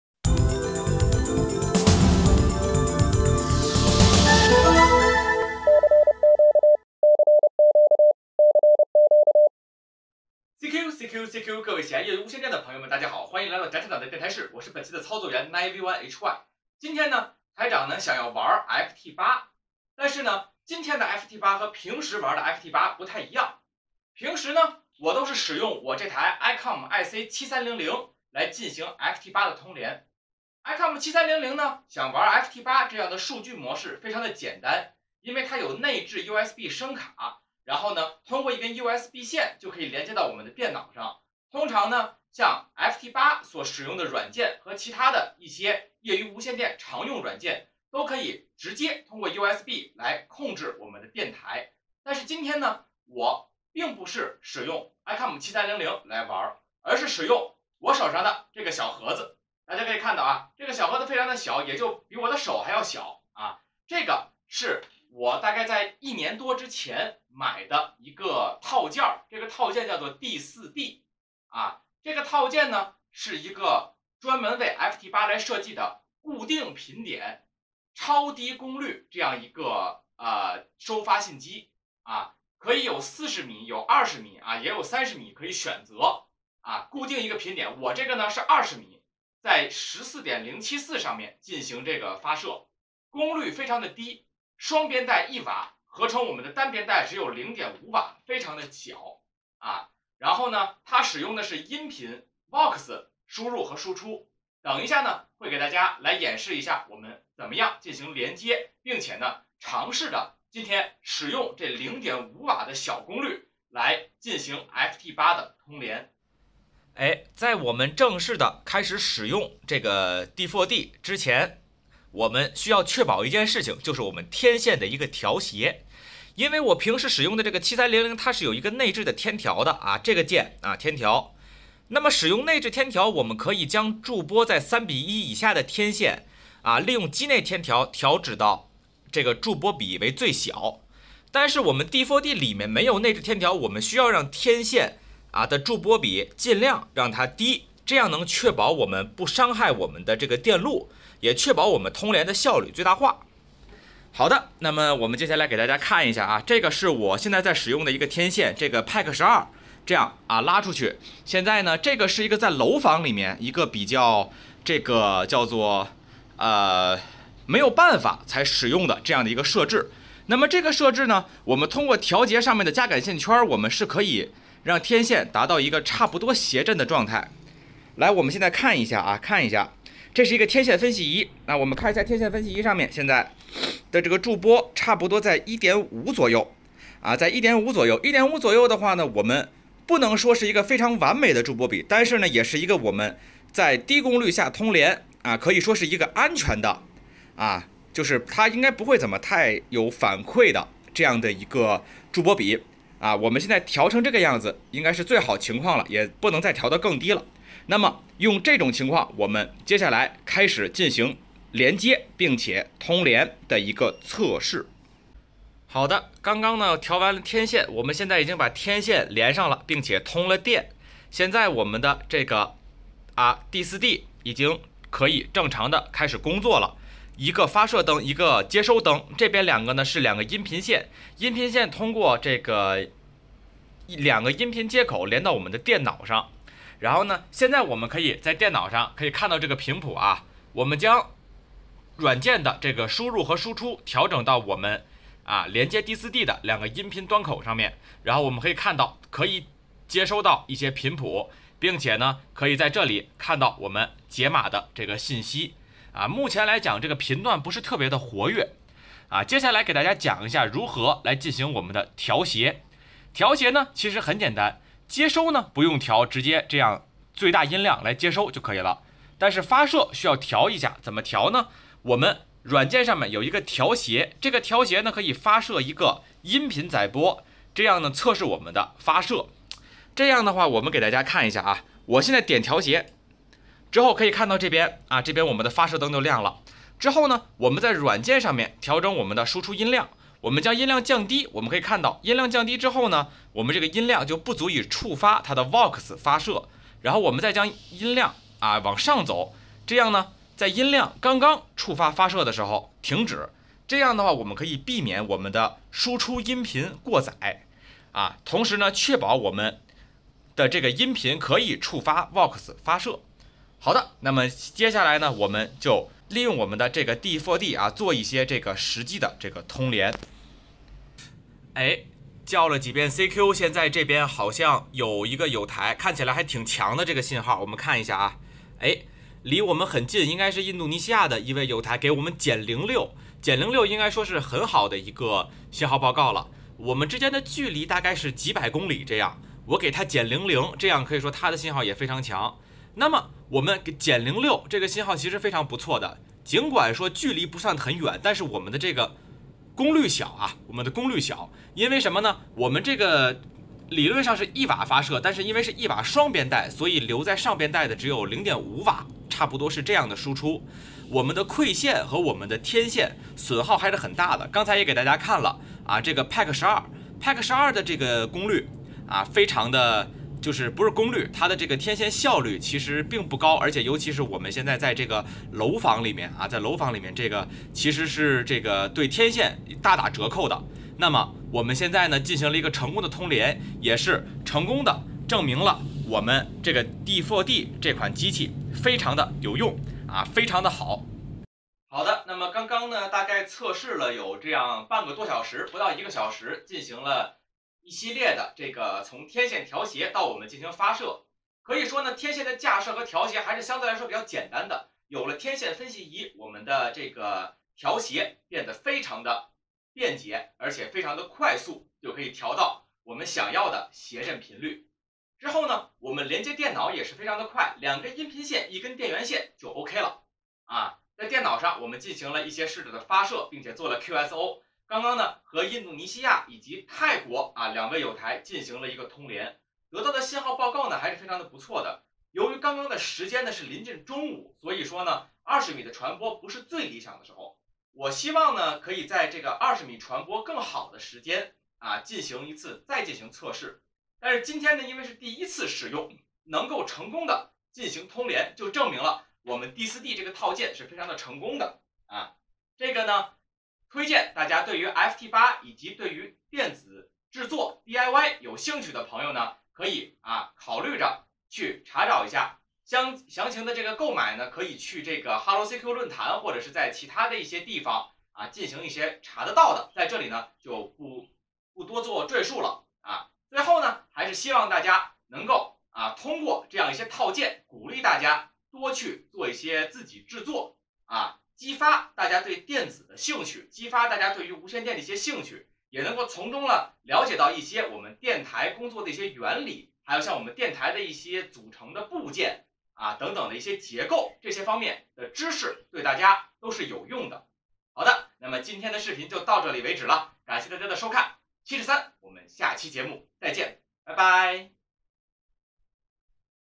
【宅台长】0.5瓦微功率FT8套件通联测试
一年前做的D4D套件，焊好了一直没有测试发射，今天拿来试一下通联，看看0.5瓦能不能通到友台，结果还真是挺开心的。

B站/西瓜视频/YouTube：宅台长
微信公众号：OurRadioShack
微博：@火腿宅台长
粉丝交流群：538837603
希望大家多多支持我们的频道！73！